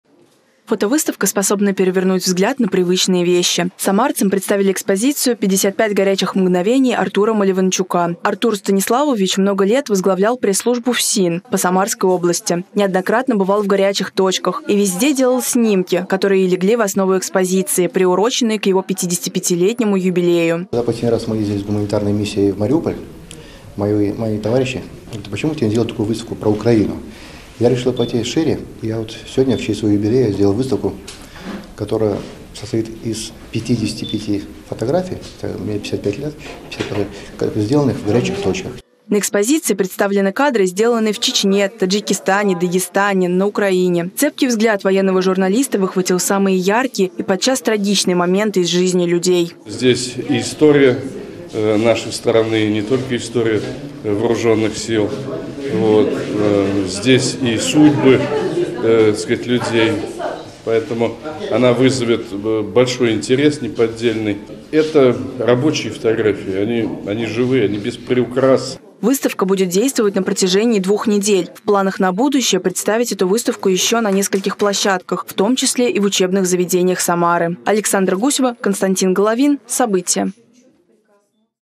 В Самаре открылась выставка "55 "горячих" мгновений Артура Маливанчука"
Фотография удивительное явление. Она позволяет не только запечатлеть мгновения и эмоции, но и передать их другим людям. В Самарском Доме журналистов состоялось открытие необычной фотовыставки - подробности в сюжете Александры Гусевой.